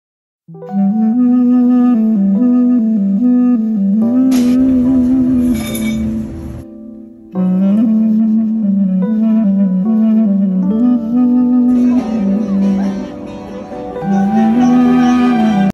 Visiting the kebap place Anthony Bourdain went to, Durumzade! Order Adana Durum for heavenly lamb!
Visiting the kebap place Anthony Bourdain went to : Durumzade!

Order Adana Durum Special meat! 35 TL - noban for generous portion roasted lamb and beef!

Tasty, juicy and yummy! One of the best street food in Istanbul.